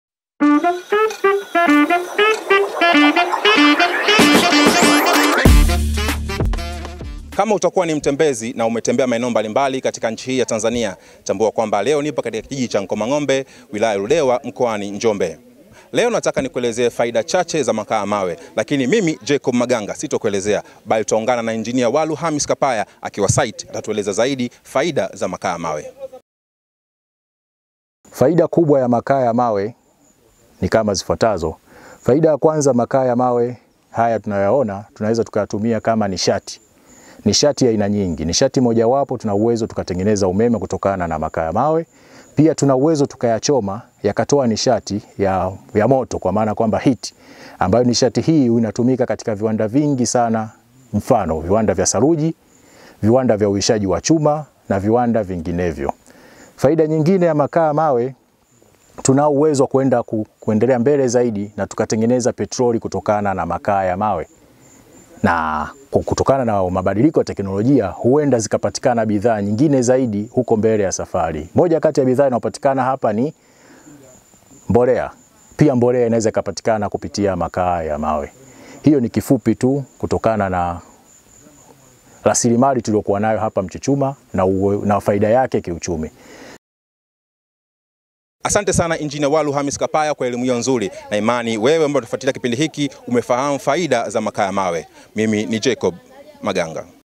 MAKAA YA MAWE NA FAIDA ZAKE KWA UCHUMI
Shirika la Taifa la MAendeleo (NDC), linamiliki kitalu cha uchimbaji makaa ya mawe, Ludewa mkoani Njombe.

Makaa ya Mawe ni nishati muhimu kwa uchumi wa nchi, karibu ujifunze.